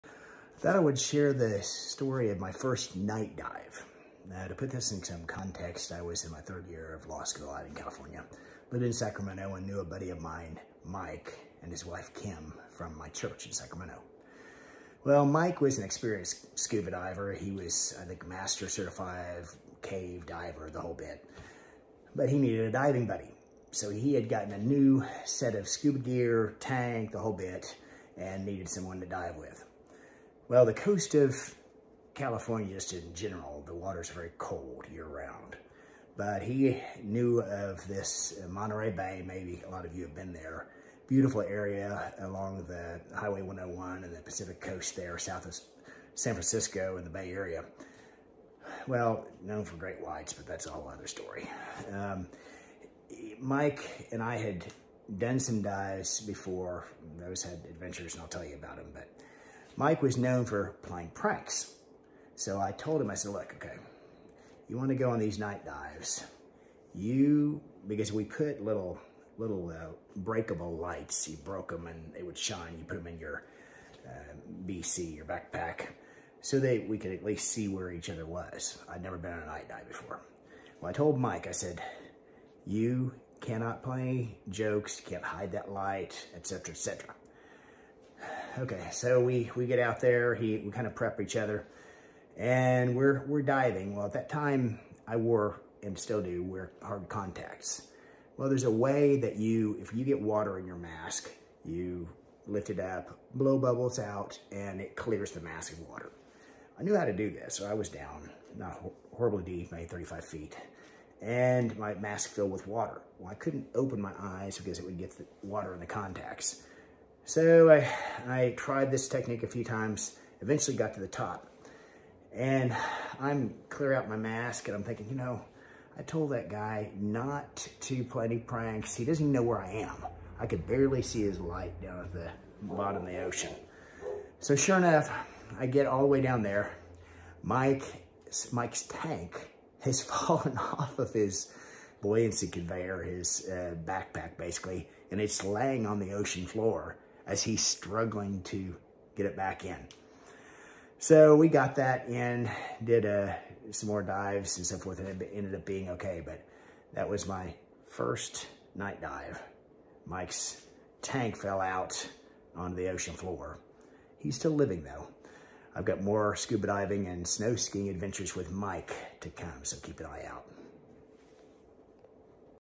Danger and unexpected happenings on my first night live! Monterey Bay, 1992
This video describes what happened on my very first night dive. Some unexpected things, for sure!